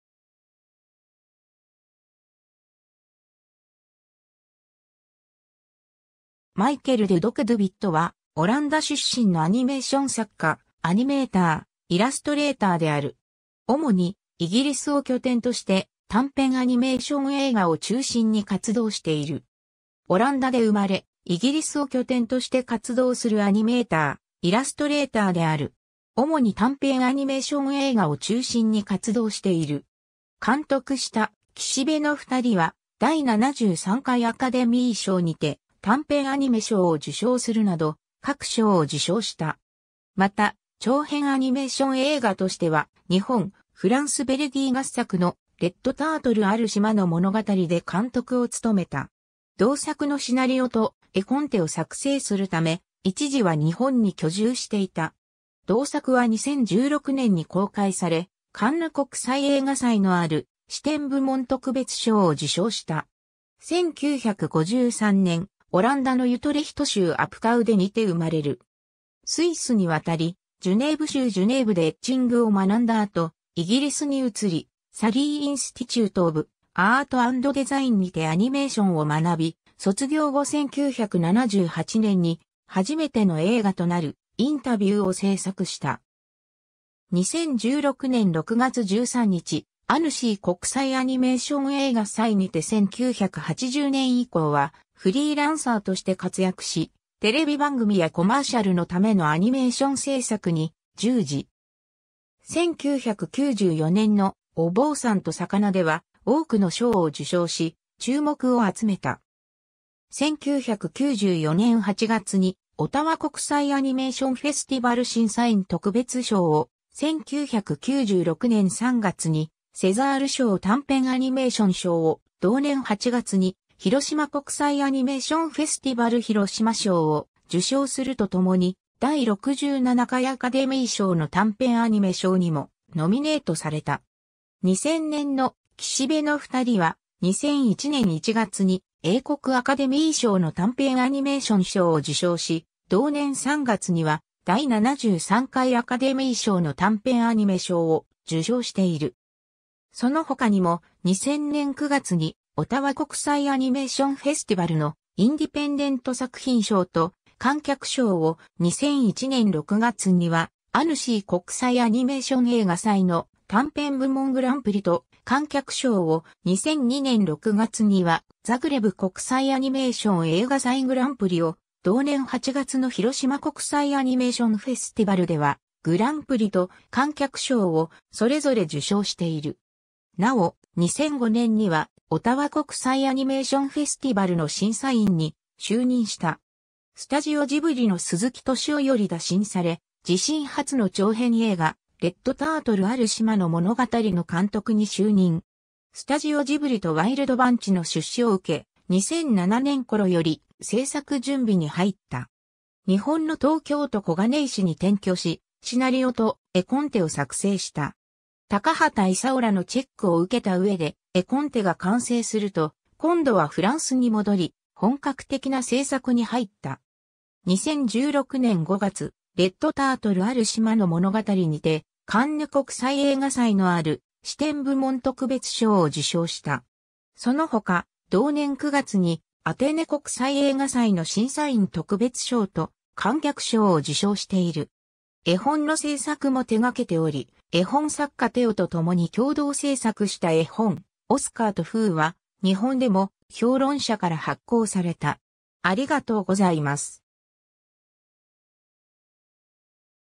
マイケル・デュドク・ドゥ・ヴィット
マイケル・デュドク・ドゥ・ヴィット（Michaël Dudok de Wit、1953年 - ）は、オランダ出身のアニメーション作家、アニメーター、イラストレーターである。
主にイギリスを拠点として、短編アニメーション映画を中心に活動している。
 オランダで生まれ、イギリスを拠点として活動するアニメーター、イラストレーターである。
主に短編アニメーション映画を中心に活動している。
監督した『岸辺のふたり』は、第73回アカデミー賞にて短編アニメ賞を受賞するなど、各賞を受賞した。
また、長編アニメーション映画としては、日本・フランス・ベルギー合作の『レッドタートル ある島の物語』で監督を務めた。
同作のシナリオと絵コンテを作成するため、一時は日本に居住していた。
同作は2016年に公開され、カンヌ国際映画祭のある視点部門特別賞を受賞した。
 1953年、オランダのユトレヒト州アプカウデにて生まれる。
スイスに渡りジュネーヴ州ジュネーヴでエッチングを学んだあとイギリスに移り、サリー・インスティテュート・オブ・アート・アンド・デザイン（現UCA芸術大学）にてアニメーションを学び、卒業後1978年に初めての映画となる『インタビュー』を制作した。
 2016年6月13日、アヌシー国際アニメーション映画祭にて 1980年以降はフリーランサーとして活躍し、テレビ番組やコマーシャルのためのアニメーション制作に従事。
1994年の『お坊さんと魚』では、多くの賞を受賞し注目を集めた。
1994年8月にオタワ国際アニメーションフェスティバル審査員特別賞を、1996年3月にセザール賞短編アニメーション賞を、同年8月に広島国際アニメーションフェスティバルヒロシマ賞を受賞するとともに、第67回アカデミー賞の短編アニメ賞にもノミネートされた。
 2000年の『岸辺のふたり』は、2001年1月に英国アカデミー賞の短編アニメーション賞を受賞し、同年3月には第73回アカデミー賞の短編アニメ賞を受賞している。
そのほかにも、2000年9月にオタワ国際アニメーションフェスティバルのインディペンデント作品賞と観客賞を、2001年6月にはアヌシー国際アニメーション映画祭の短編部門グランプリと観客賞を、2002年6月にはザグレブ国際アニメーション映画祭グランプリを、同年8月の広島国際アニメーションフェスティバルではグランプリと観客賞を、それぞれ受賞している。
なお、2005年には、オタワ国際アニメーションフェスティバルの審査員に就任した。
 スタジオジブリの鈴木敏夫より打診され、自身初の長編映画『レッドタートル ある島の物語』の監督に就任。
スタジオジブリとワイルドバンチの出資を受け、2007年頃より製作準備に入った。
日本の東京都小金井市に転居し、シナリオと絵コンテを作成した。
高畑勲らのチェックを受けたうえで、絵コンテが完成すると今度はフランスに戻り、本格的な製作に入った。
2016年5月、『レッドタートル ある島の物語』にて、カンヌ国際映画祭のある視点部門特別賞を受賞した。
そのほか、同年9月にアテネ国際映画祭の審査員特別賞と観客賞を受賞している。
 絵本の制作も手掛けており、絵本作家テオとともに共同制作した絵本『オスカーとフー』は、日本でも評論社から発行された。